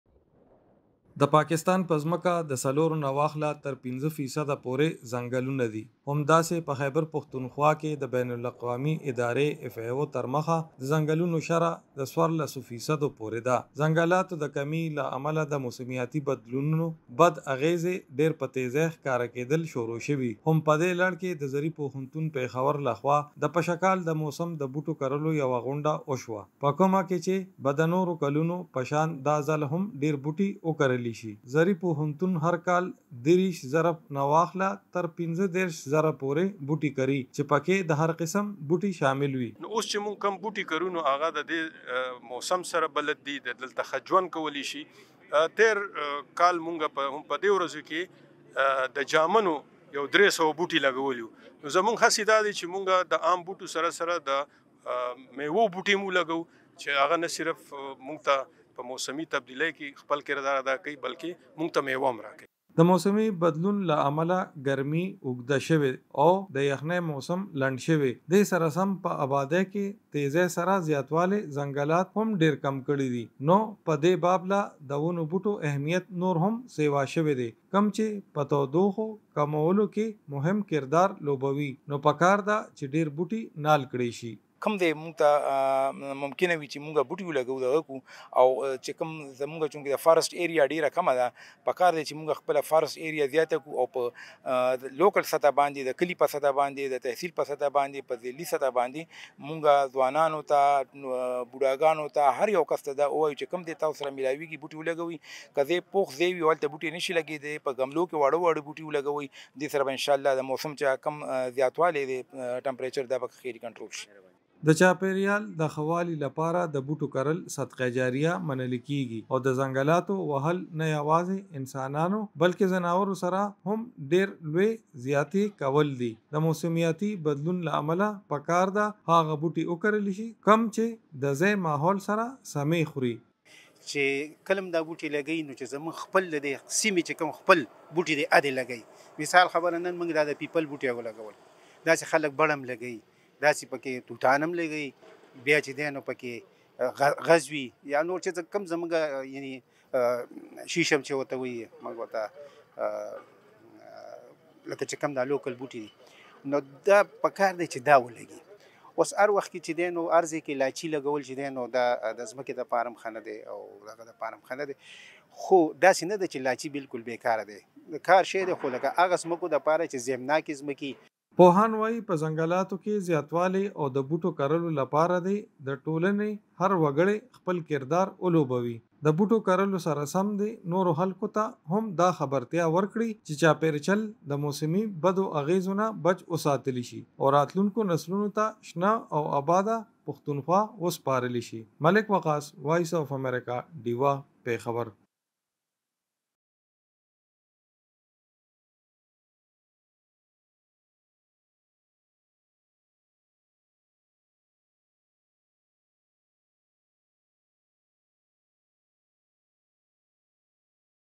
کارپوهان وايي پښتونخوا کې په موسمي بدلون قابو موندلو لپاره مهمه ده چې د ځنګلونو رقبه زياته کړی شي
کارپوهان وايي په موسمي بدلون قابو موندلو لپاره مهمه ده چې د ځنګلونو رقبه زياته کړی شي. په پاکستان کې د ځنګلونو ځمکه تر پينځه فيصده پورې ده چې ډېره کمه ده. د «پاک او شين پاکستان» د نعرې لاندې د پشکال د موسمي بوټو کرنې يوه غونډه د پېښور په زرعي پوهنتون کې شوې، تفصیل د ملک وقاص په رپورټ کې: